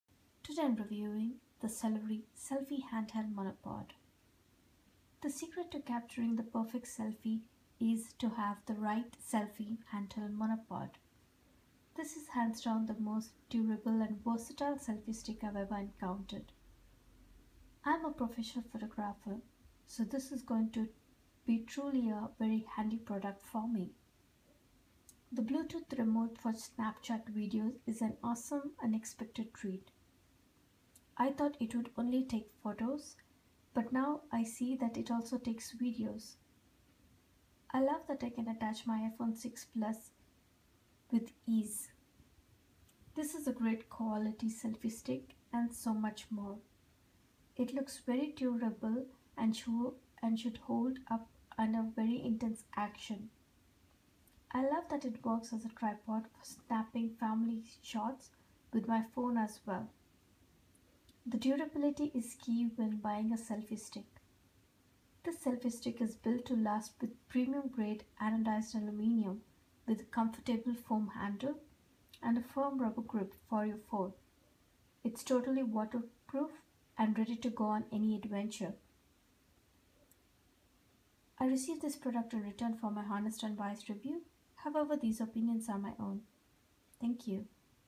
Cellery Selfie Stick
Selfie Stck with FREE Travel Bag! - The Secret of Capturing the Perfect Selfie

No Bluetooth pairing required - Simply Plug and Snap!Compatible With
- Apple iPhone 4, 5, 6 (4.7), 6 Plus (5.5)
- Samsung Galaxy S3, S4, S5, S6, Edge and many more!
- GoPro cameras

- Made from high quality materials, making it durable and resistant for all conditions
- Rubber Handle for Extra Grip and special attention to ergonomics
- Battery Free - No need to worry about it being charged
- Extends from 23cm to 97cm with selection of 5 adjustable heights.

 I received this product in exchange for my honest unbiased review. However the opinions are my own.